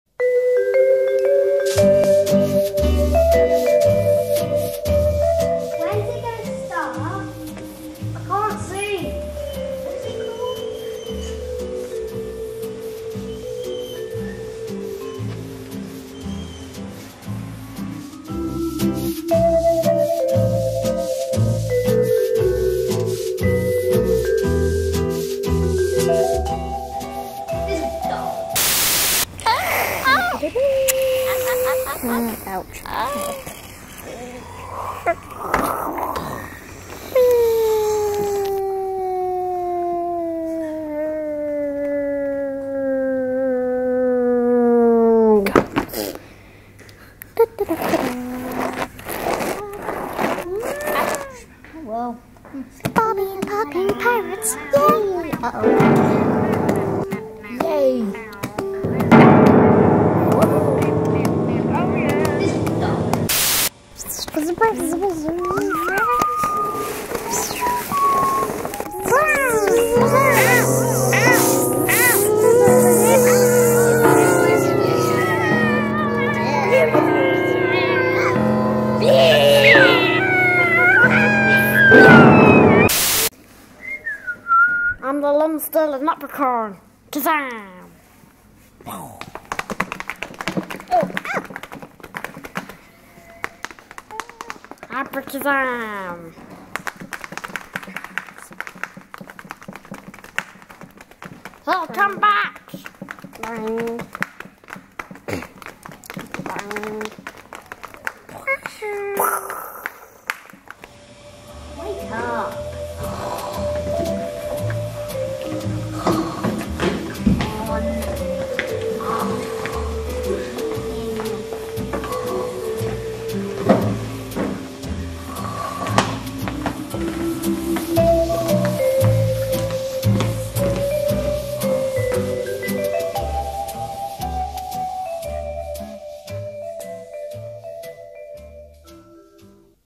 Chaos At The Cinema
Short film where the cinema audience decide they would rather watch their own home-made animations, although there seems to be some dispute over which animation to watch.. Made on a 2-day animation workshop by 8 young talented trainee animators as part of West Sussex County Council's Enrichment programme. Facilitated by Rosie Holmes and Helen Goodwin, at Millais School in Horsham, in June 2011.